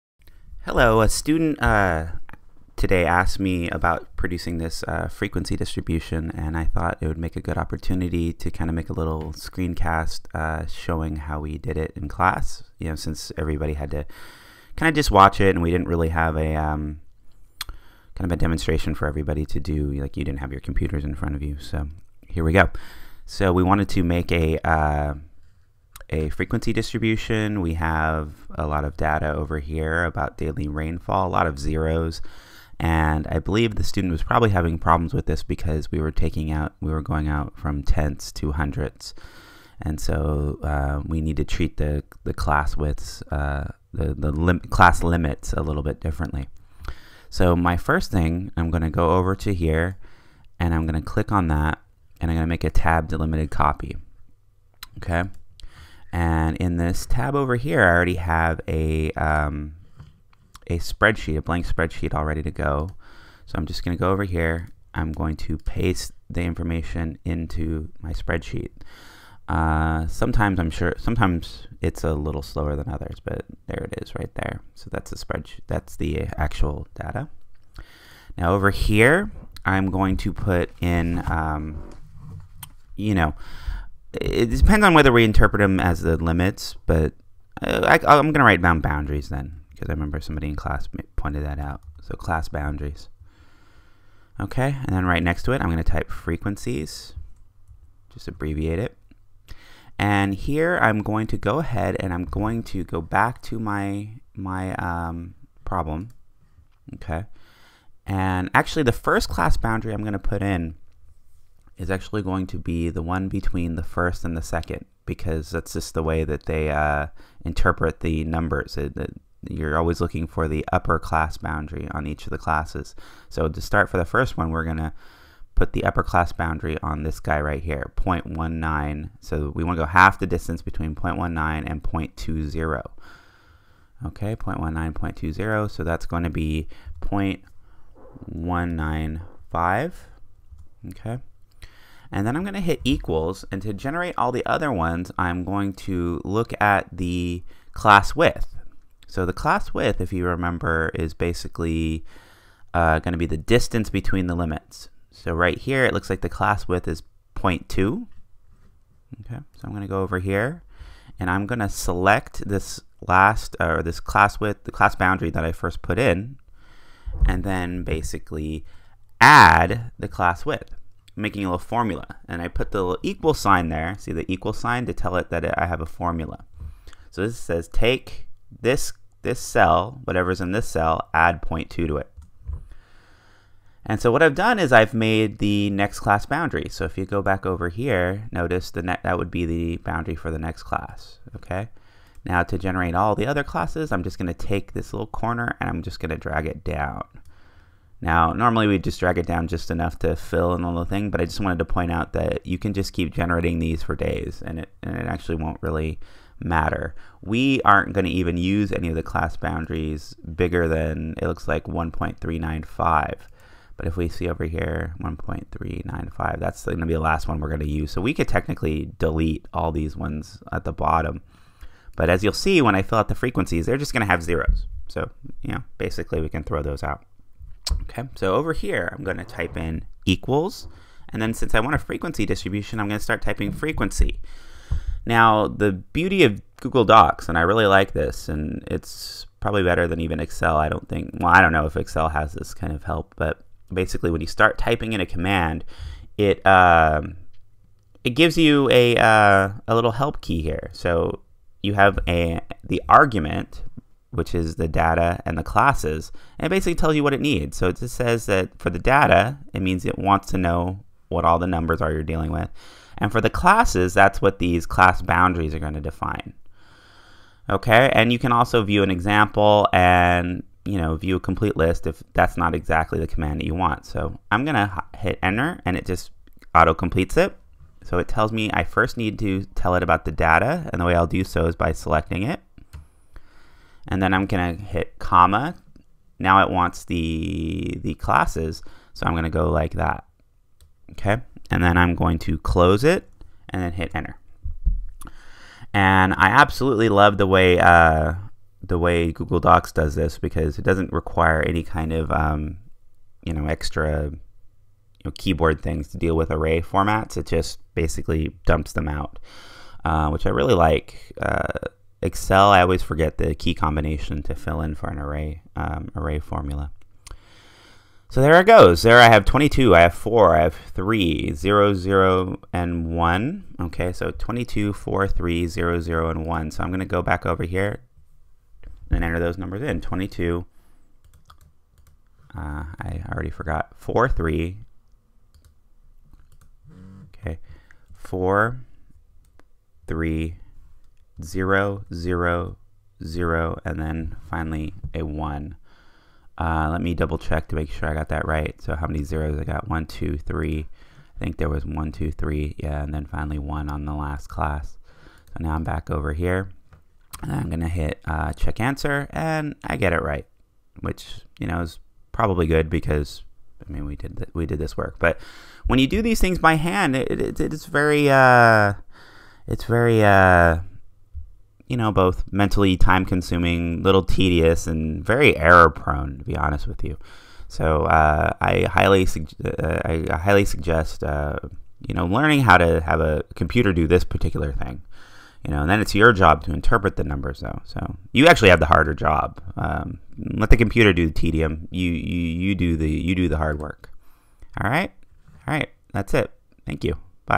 Frequency Distributions using Google Docs
This video is about creating a frequency distribution using google docs and how to import data from My Math Lab.